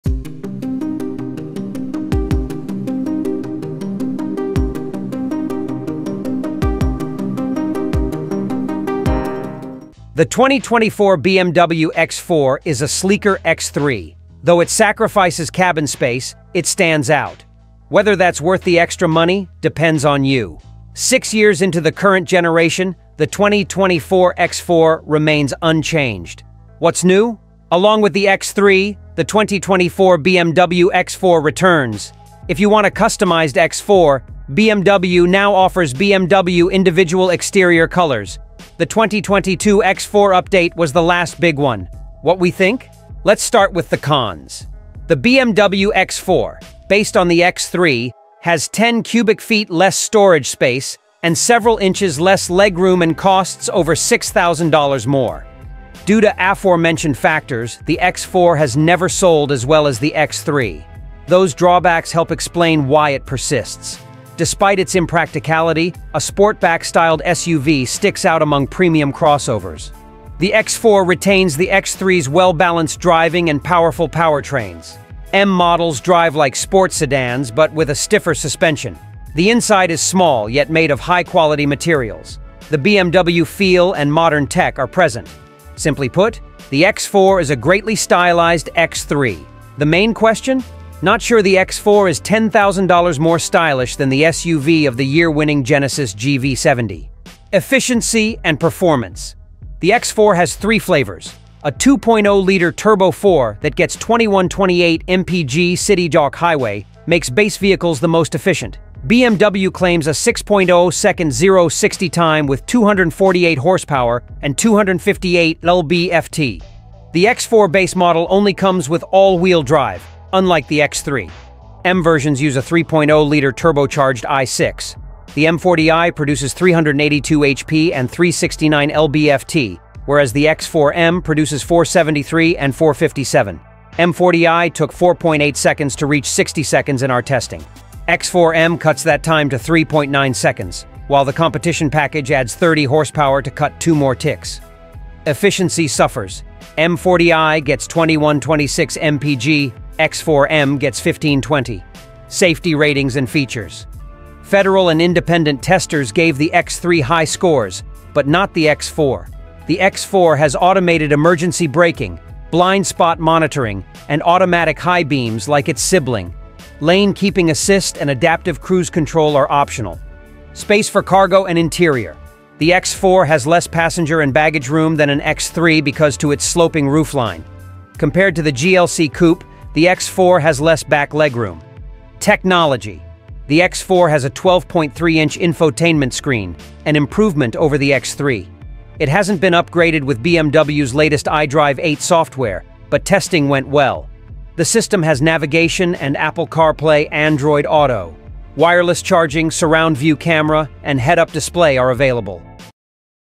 2024 - 2025 BMW X4 : The Ultimate SUV Upgrade Unveiled!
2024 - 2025 BMW X4 : The Ultimate SUV Upgrade Unveiled!

The 2024 BMW X4 is a sleeker X3. Though it sacrifices cabin space, it stands out. Whether that's worth the extra money depends on you. Six years into the current generation, the 2024 X4 remains unchanged.

What's New?

Along with the X3, the 2024 BMW X4 returns. If you want a customised X4, BMW now offers BMW Individual exterior colours. The 2022 X4 update was the last big one.

What We Think?

Let's start with the cons. The BMW X4, based on the X3, has 10 cubic feet less storage space and several inches less legroom and costs over $6,000 more.

Due to aforementioned factors, the X4 has never sold as well as the X3. Those drawbacks help explain why it persists. Despite its impracticality, a sportback-styled SUV sticks out among premium crossovers.